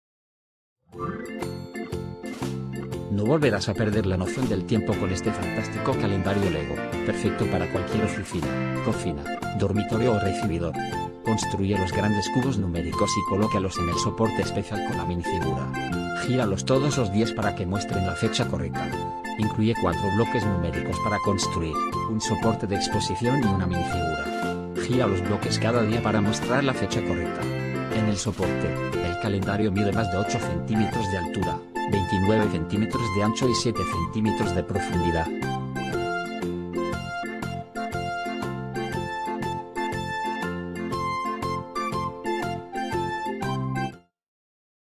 Lego Calendario de ladrillos LEGO Iconic 40172: Revisión
No volverás a perder la noción del tiempo con este fantástico calendario LEGO, perfecto para cualquier oficina, cocina, dormitorio o recibidor. Construye los grandes cubos numéricos y colócalos en el soporte especial con la minifigura. ¡Gíralos todos los días para que muestren la fecha correcta! Incluye 4 bloques numéricos para construir, un soporte de exposición y una minifigura. Gira los bloques cada día para mostrar la fecha correcta. En el soporte, el calendario mide más de 8 cm de altura, 29 cm de ancho y 7 cm de profundidad.